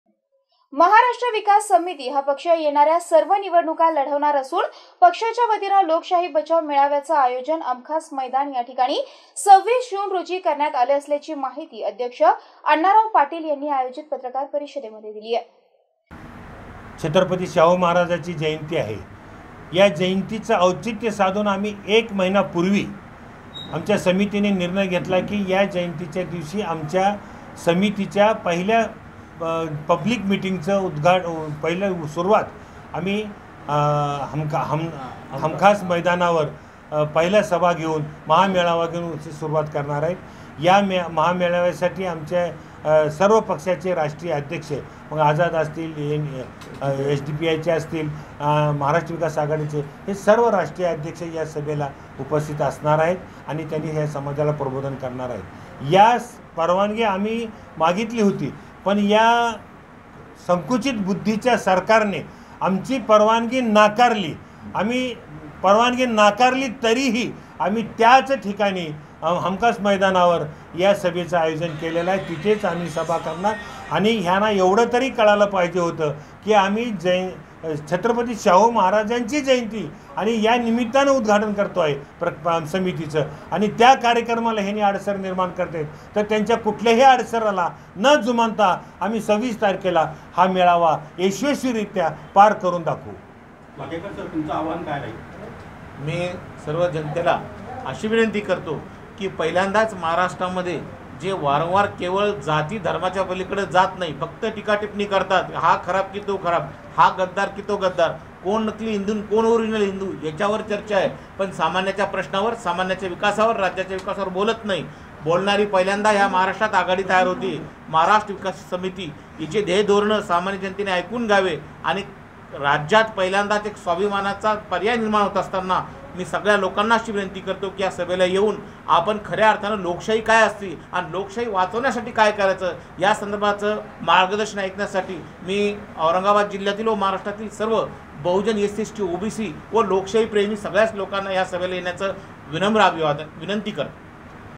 MCN NEWS| महाराष्ट्र विकास समिती येणाऱ्या सर्व निवडणुका ताकदीने लढविणार
महाराष्ट्र विकास समिती येणाऱ्या सर्व निवडणुका ताकदीने लढविणार आहे. कुठलीही जात, धर्म आणि लिंगभेद न करणारा हा पक्ष असून यात सर्व जातीधर्माचे लोक एकत्र आहेत. पक्षातर्फे लोकशाही बचाव महामेळाव्याचे आयोजन आमखास मैदान येथे 26 जून रोजी करण्यात आल्याची माहिती अध्यक्ष अन्नाराव पाटील यांनी आयोजित पत्रकार परिषदेत दिलीय.